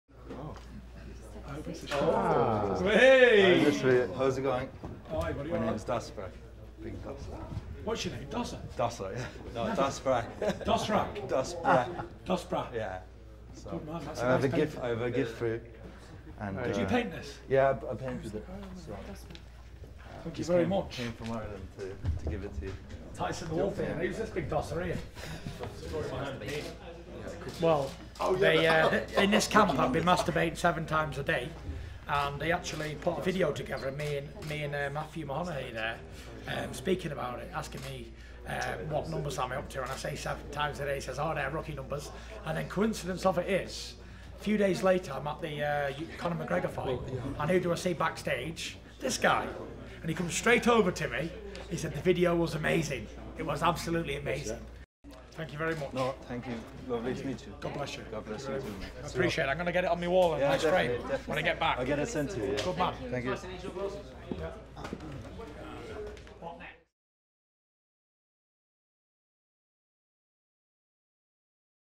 "Who's that big dosser?!" 😂 Tyson Fury meets Dosbrak and is presented with amazing artwork
Tyson Fury is presented with an illustration created by Irish artist Dosbrak, referencing his famous quote about masturbating seven times a day in the build up to his rematch with Deontay Wilder.